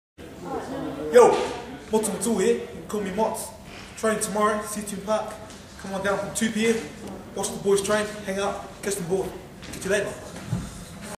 Motu Matu'u Invites You to Seatoun!
The Hurricanes have a public training in Seatoun tomorrow from 2pm. The boys did a bit of filming today and recorded these messages to invite you along!